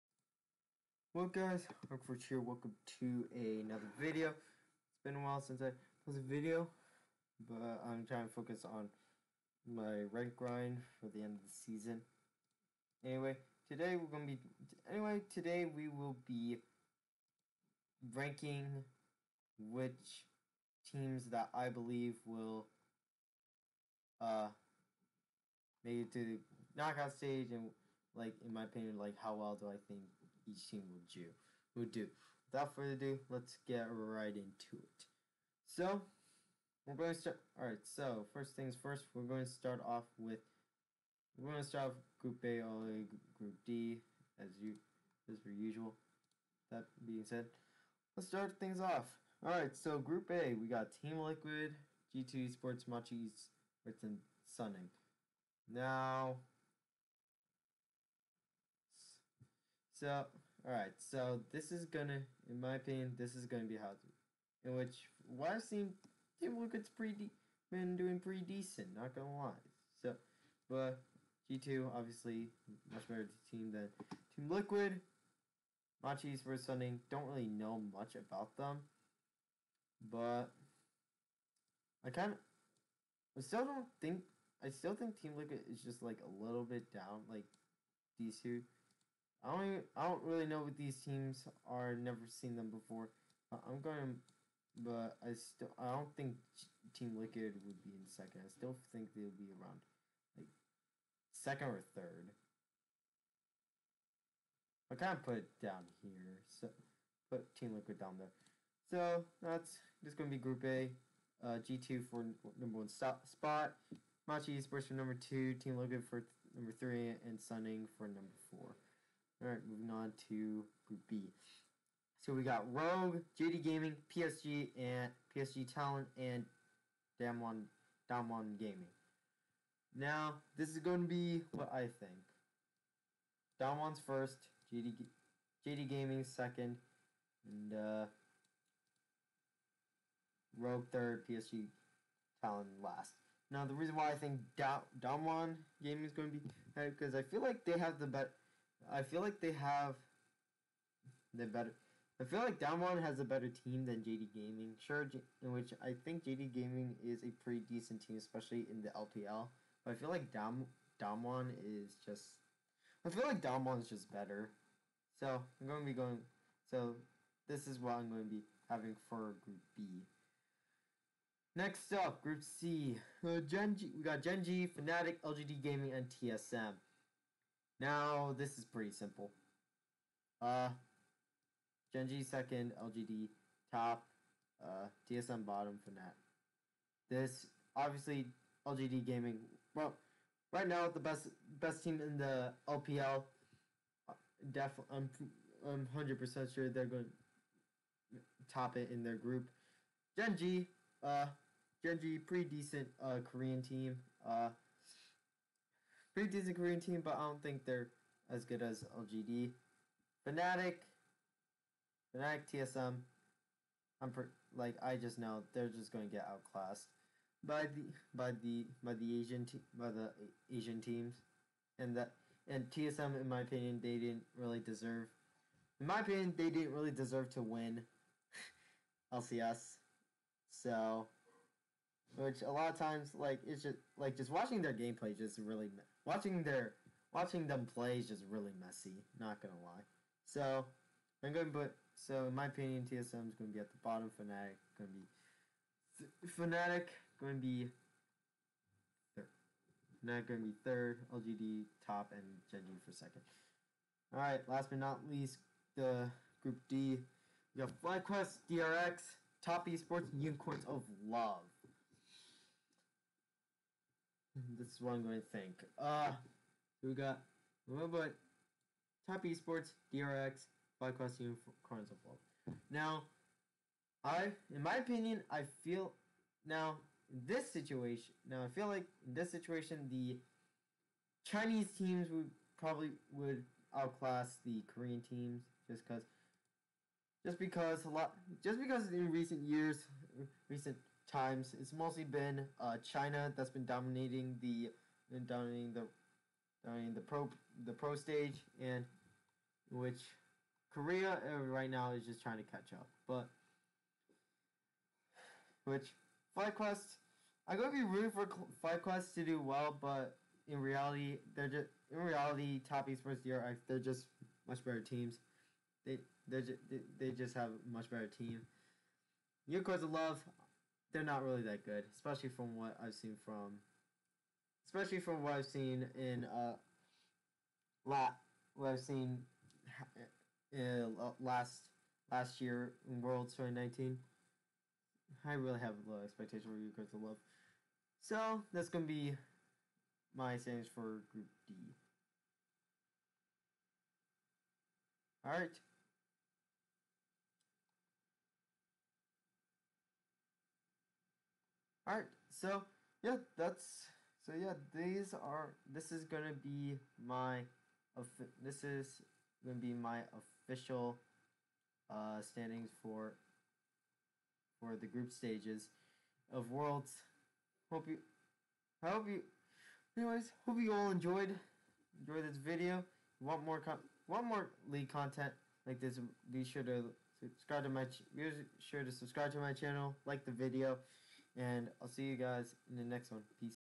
Worlds Team Rankings
My views and opinions on which team will make it to the knockout stage at Worlds 2020.
Live stream every Saturday.